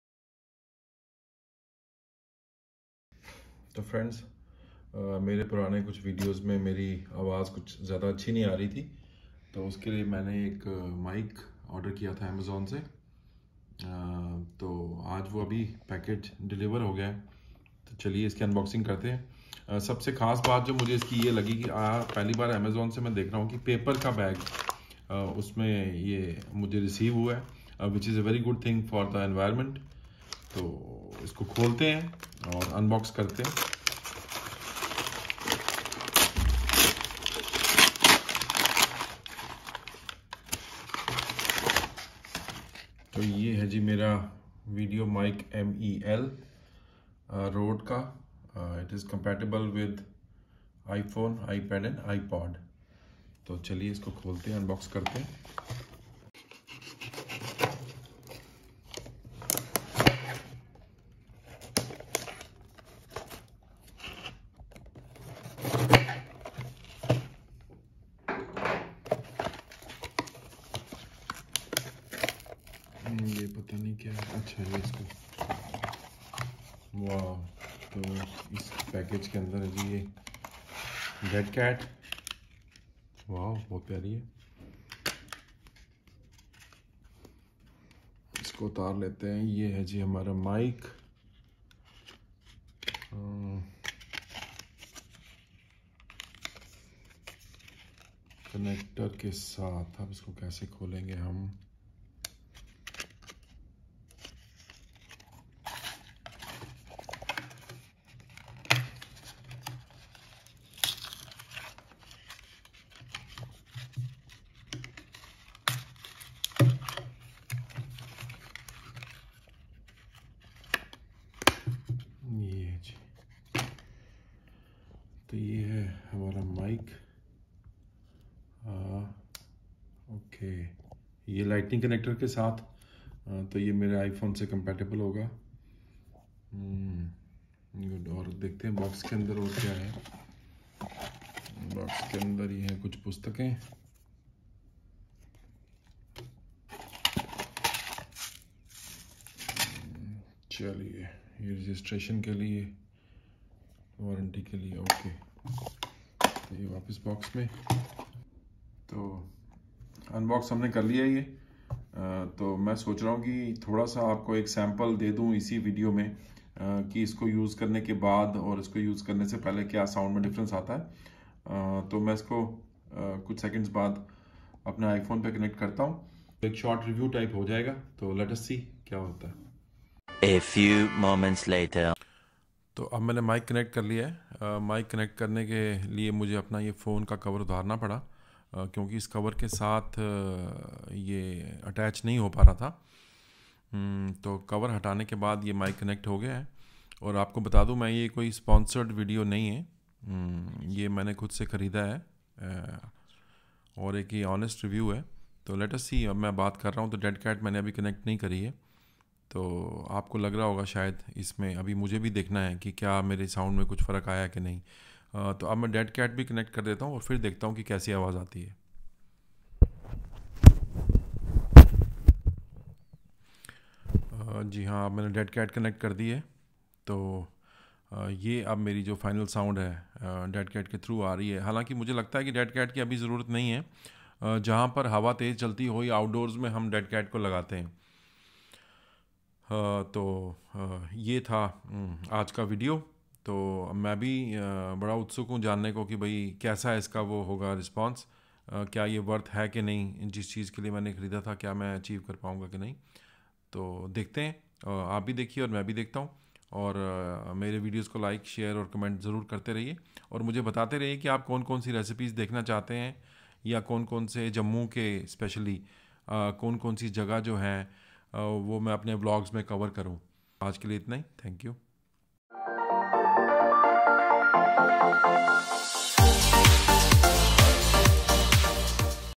Rode VideoMic Me-L unboxing and short review. Best mic for iphone
Rode VideoMic Me-L directional microphone for iPad, iPhone with lighting connector.